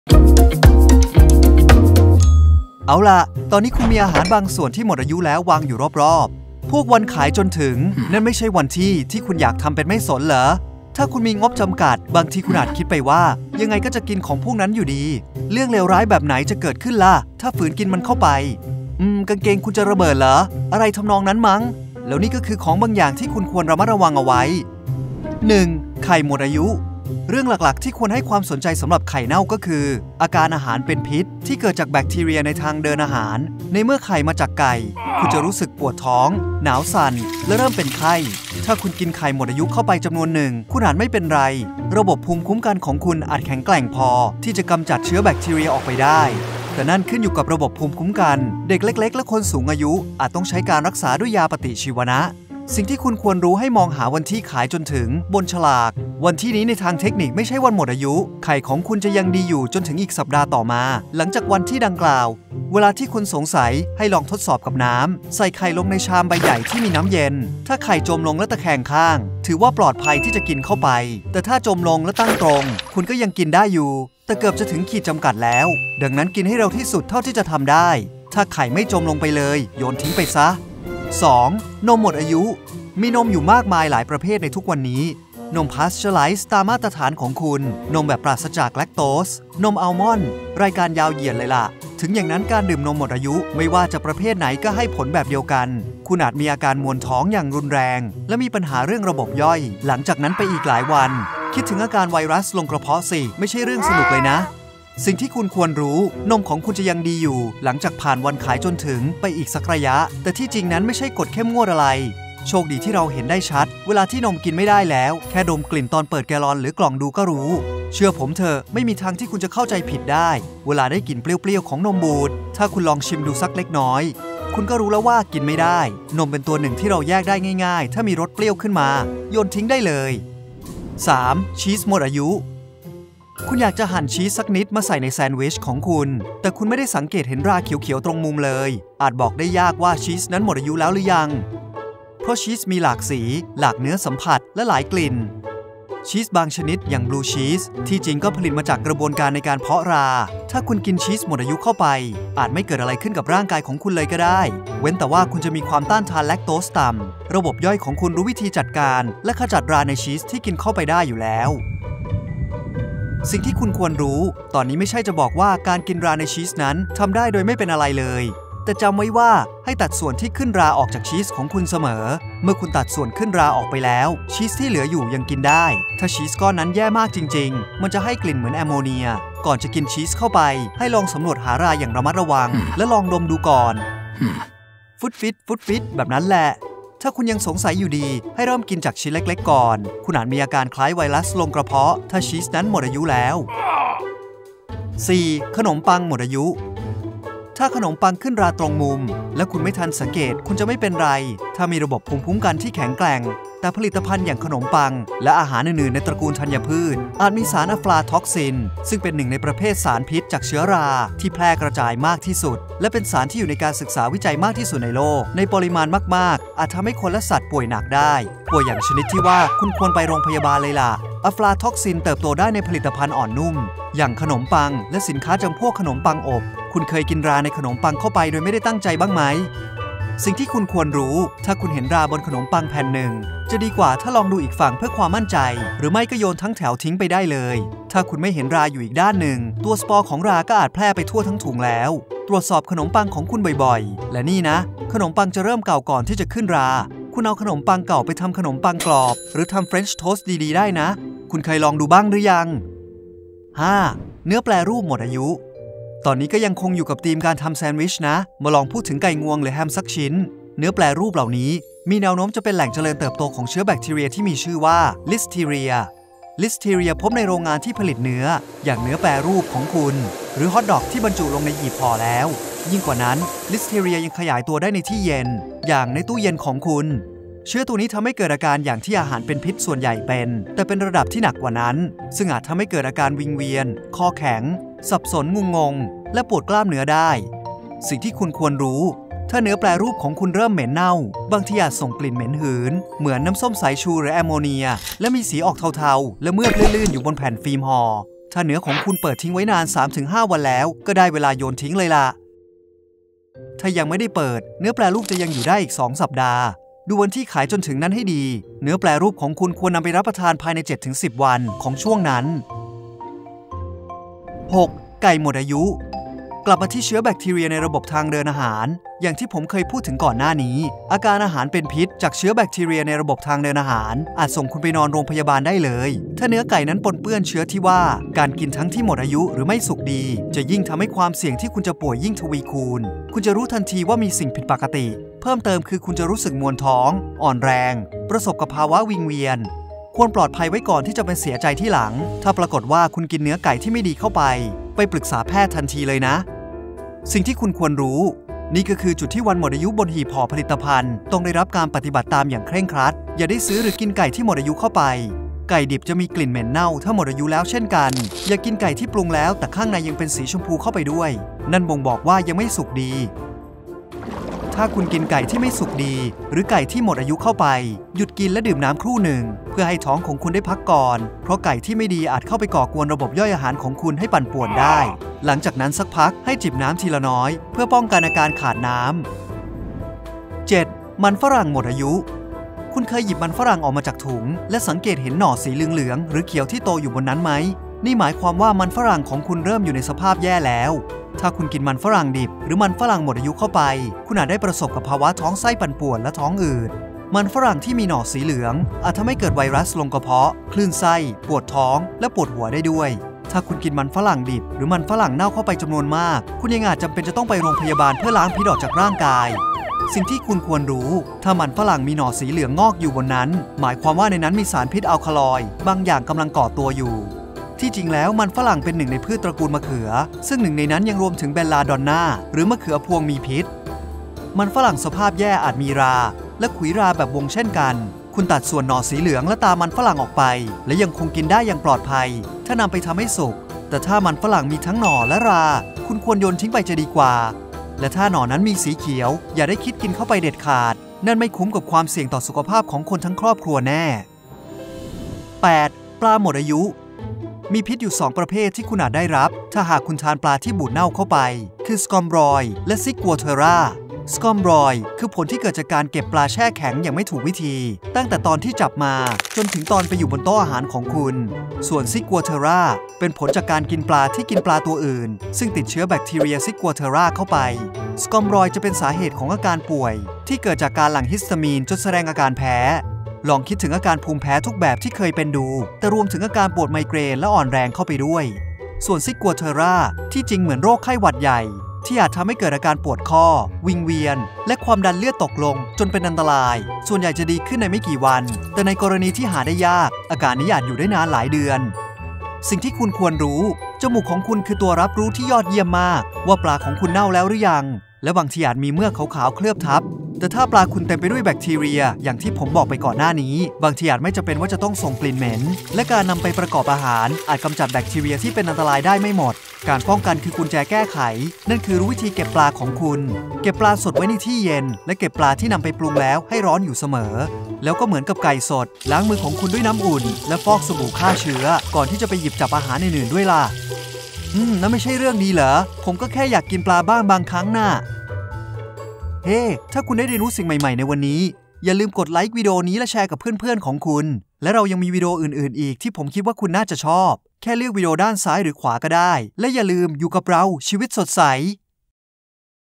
จะเกิดอะไรขึ้นถ้าคุณกินไข่หมดอายุเข้าไป
เอาล่ะ...ตอนนี้คุณมีอาหารบางส่วนที่หมดอายุแล้ววางอยู่รอบๆ. พวกวัน “ขายจนถึง” นั่น ไม่ใช่วันที่ที่คุณอยากทำเป็นไม่สนเหรอ? ถ้าคุณมีงบจำกัด บางทีคุณอาจคิดไปว่า ยังไงก็จะกินของพวกนั้นอยู่ดี. เรื่องเลวร้ายแบบไหนจะเกิดขึ้นล่ะ? แล้วนี่ก็คือของบางอย่างที่คุณควรระมัดระวังเอาไว้

อย่างเช่น เนื้อไก่ นี่ก็คือจุดที่วันหมดอายุบนหีบห่อผลิตภัณฑ์ ต้องได้รับการปฏิบัติตามอย่างเคร่งครัด ถ้าคุณกินไก่ที่ไม่สุกดีหรือไก่ที่หมดอายุเข้าไป หยุดกินและดื่มน้ำครู่หนึ่ง เพื่อให้ท้องของคุณได้พักก่อน เพราะไก่ที่ไม่ดีอาจเข้าไปก่อกวนระบบย่อยอาหารของคุณให้ปั่นป่วนได้ แล้วผลิตภัณฑ์อื่นๆ ล่ะ?

TIMESTAMPS:
ไข่หมดอายุ 0:23
นมหมดอายุ1:19
ชีสหมดอายุ 2:07
ขนมปังหมดอายุ 3:12
เนื้อแปรรูปหมดอายุ 4:10
ไก่หมดอายุ 5:10
มันฝรั่งหมดอายุ 6:15
ปลาหมดอายุ 7:20

สรุป:
- เรื่องหลักๆ ที่ควรให้ความสนใจสำหรับไข่เน่าก็คือ อาการอาหารเป็นพิษ ที่เกิดจากแบคทีเรียในทางเดินอาหาร ในเมื่อไข่มาจากไก่ คุณจะรู้สึกปวดท้อง หนาวสั่น และเริ่มเป็นไข้
- การดื่มนมหมดอายุ ไม่ว่าจะประเภทไหนก็ให้ผลแบบเดียวกัน คุณอาจมีอาการมวนท้องอย่างรุนแรง และมีปัญหาเรื่องระบบย่อยหลังจากนั้นไปอีกหลายวัน
- ถ้าคุณกินชีสหมดอายุเข้าไป อาจไม่เกิดอะไรขึ้นกับร่างกายของคุณเลยก็ได้ เว้นแต่ว่าคุณจะมีความต้านทานแลคโตสต่ำ ระบบย่อยของคุณรู้วิธีจัดการ และขจัดราในชีสที่กินเข้าไปได้อยู่แล้ว
- ตอนนี้ไม่ใช่จะบอกว่า การกินราในชีสนั้นทำได้โดยไม่เป็นอะไรเลย แต่จำไว้ว่า ให้ตัดส่วนที่ขึ้นราออกจากชีสของคุณเสมอ. เมื่อคุณตัดส่วนขึ้นราออกไปแล้ว ชีสที่เหลืออยู่ยังกินได้ ถ้าชีสก้อนนั้นแย่มากจริงๆ แล้ว มันจะให้กลิ่นเหมือนแอมโมเนีย
- ผลิตภัณฑ์อย่างขนมปัง และอาหารอื่นๆ ในตระกูลธัญพืช อาจมีสารอะฟลาท็อกซิน ซึ่งเป็นหนึ่งในประเภทสารพิษจากเชื้อรา ที่แพร่กระจายมากที่สุด และเป็นสารที่อยู่ในการศึกษาวิจัยมากที่สุดในโลก
- เนื้อแปรรูปเหล่านี้ มีแนวโน้มจะเป็นแหล่งเจริญเติบโตของเชื้อแบคทีเรียที่มีชื่อว่า ลิสทีเรีย ทำให้เกิดอาการอย่างที่อาหารเป็นพิษส่วนใหญ่เป็น แต่เป็นระดับที่หนักกว่านั้น ซึ่งอาจทำให้เกิดอาการวิงเวียน คอแข็ง สับสนงุนงง และปวดกล้ามเนื้อได้
- อาการอาหารเป็นพิษ จากเชื้อแบคทีเรียในระบบทางเดินอาหาร อาจส่งคุณไปนอนโรงพยาบาลได้เลย ถ้าเนื้อไก่นั้นปนเปื้อนเชื้อที่ว่า การกินทั้งที่หมดอายุหรือไม่สุกดี จะยิ่งทำให้ความเสี่ยงที่คุณจะป่วยยิ่งทวีคูณ
- คุณเคยหยิบมันฝรั่งออกมาจากถุง และสังเกตเห็นหน่อสีเหลืองๆ หรือสีเขียวที่โตอยู่บนนั้นไหม? นี่หมายความว่า มันฝรั่งของคุณเริ่มอยู่ในสภาพแย่แล้ว
- มีพิษอยู่สองประเภทที่คุณอาจได้รับ ถ้าหากคุณทานปลาที่บูดเน่าเข้าไป คือสคอมบรอยด์ และ ซิกัวเทอร่า.
- สคอมบรอยด์ คือผลที่เกิดจากการเก็บปลาแช่แข็งอย่างไม่ถูกวิธี ตั้งแต่ตอนที่จับมา จนถึงตอนไปอยู่บนโต๊ะอาหารของคุณ ส่วนซิกัวเทอร่า เป็นผลจากการกินปลาที่กินปลาตัวอื่น ซึ่งติดเชื้อแบคทีเรียซิกัวเทอร่าเข้าไป